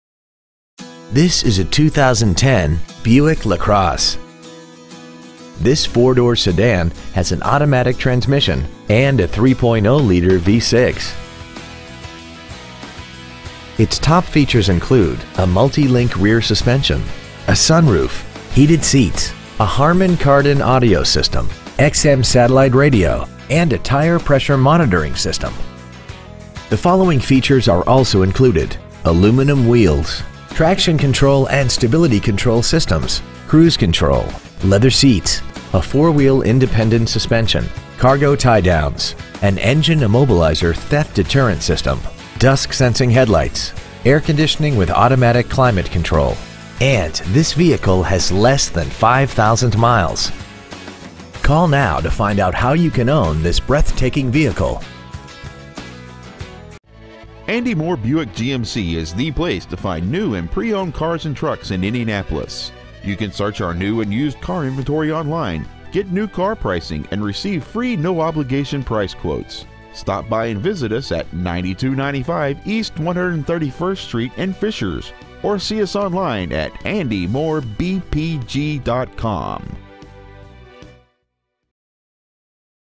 2010 Buick LaCrosse Indianapolis IN 46038
Year : 2010
 Make : Buick
 Model : LaCrosse
 Engine : 3.0L V6 24V GDI DOHC
 Trans . : 6-Speed Automatic
 Exterior : Red Jewel Tintcoat
 Miles : 4,954
 Interior : Cocoa / Light Cashmere
 Stock : B3112A

 9295 E. 131st Street